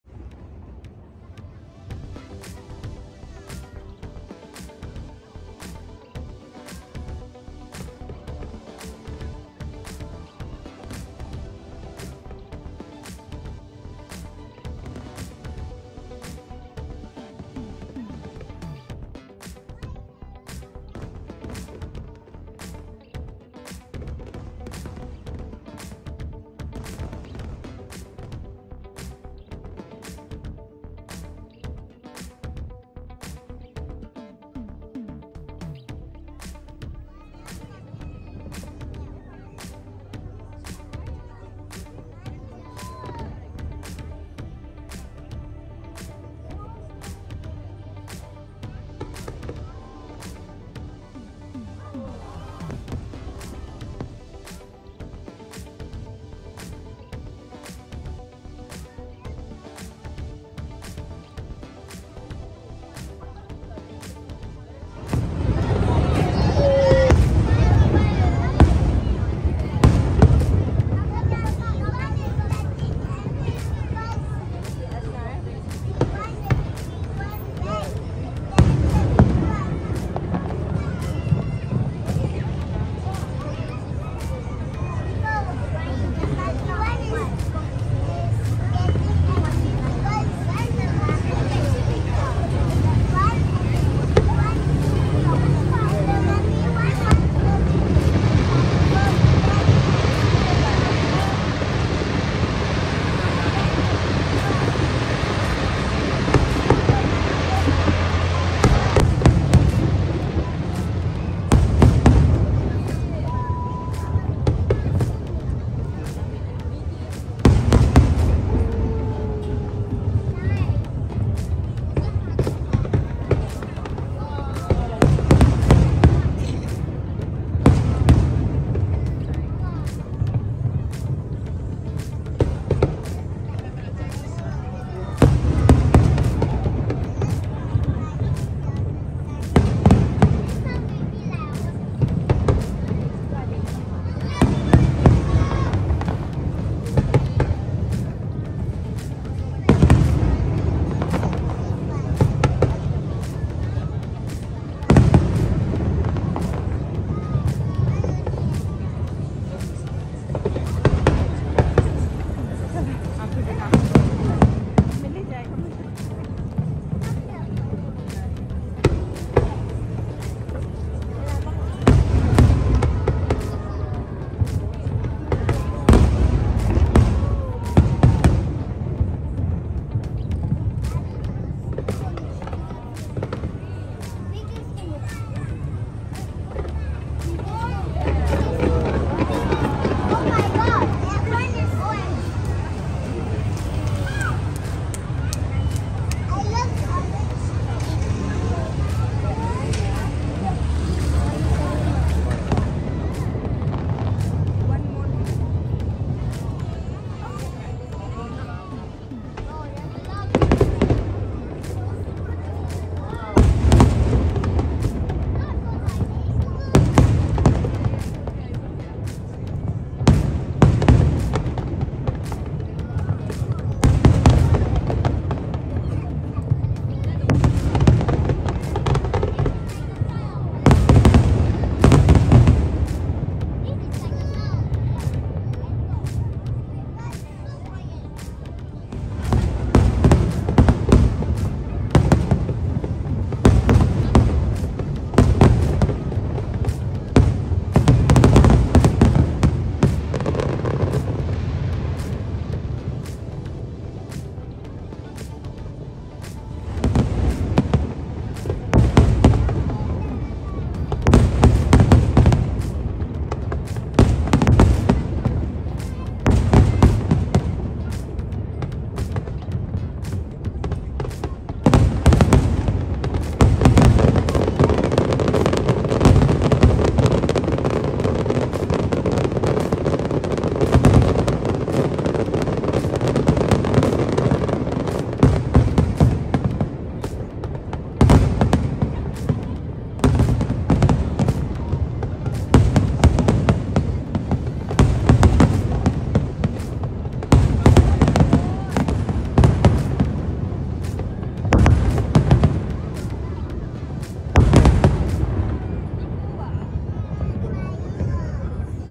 Lighting up the skies in celebration of Singapore’s 59th National Day! 🇸🇬🎆
Hi everyone today showing you fireworks during 59th National Day singapore.